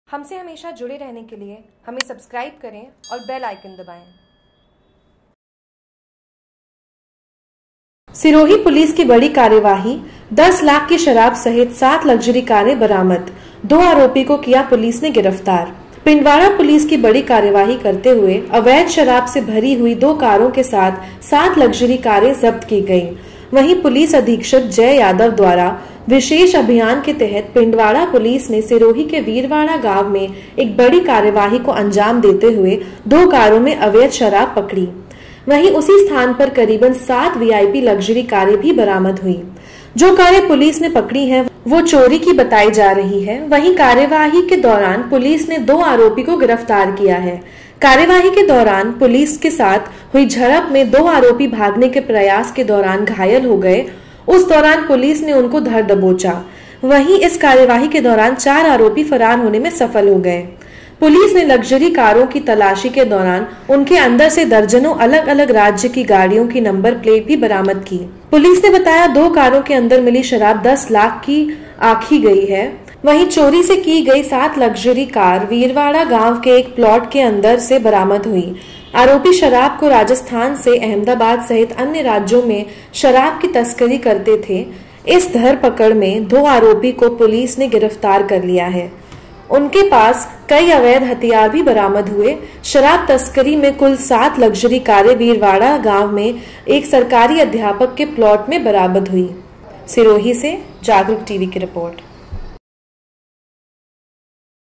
JAGRUK TV : सिरोही : 10 लाख कि शराब सहित 7 लग्जरी कारे बरामद 2 आरोपी गिरफतार
सिरोही - पिंडवाड़ा पुलिस की बड़ी कार्रवाई करते हुए  अवैध शराब से भरी हुई दो कारो के साथ 7 लग्जरी कारें जब्त कि गई। वही पुलिस अधिक्षक जय यादव द्वारा विशेष अभियान के तहत पिण्डवाड़ा पुलिस ने सिरोही के वीरवाड़ा गांव में एक बड़ी कार्रवाही को अंजाम देते हुए दो कारों में अवैध शराब से भरी हुई पकड़ी गई। वही उसी स्थान पर करीबन 7 वीआईपी लग्जरी कारे भी बराबद हुई। जो कारे पुलिस ने पकड़ी है वो चोरी कि बताई जा रही है वही कार्रवाही के दौरान पुलिस ने दो आरोपी को गिरफतार किया है। कार्रवाही के दौरान पुलिस के साथ हुई झडप में दो आरोपी भागने के प्रायास के दौरान घायल हो गए उस दौरान पुलिस ने उनको धरदबोचा वही इस कार्रवाही के दौरान 4 आरोपी फरार होने में सफल हो गए।  पुलिस ने लग्जरी कारो कि तलासी के दौरान उनके अंदर से दर्जनों अलग अलग राज्य कि गाडियों कि नंबर प्लेट भी बराबद हुई है।

पुलिस ने बताया दो कारो के अंदर मिली शराब दस लाख कि आखी गई है। वही चोरी से कि गई 7 लग्जरी कार वीरवाड़ा गांव के एक प्लाट के अंदर से बराबद हुई। आरोपी शराब को राजस्थान से अहबदाब सहित अन्य राज्यों में शराब की तस्करी करते थे। इस धर पकड़ में दो आरोपी को पुलिस ने गिरफतार कर दिया है। उनके पास से कई अवैध हथियार भी बराबद हुए है। शराब तस्करी में कुल 7 लग्जरी कारे वीरवाड़ा गांव में एक सरकारी अध्यापक के प्लाट में बराबद हुई है।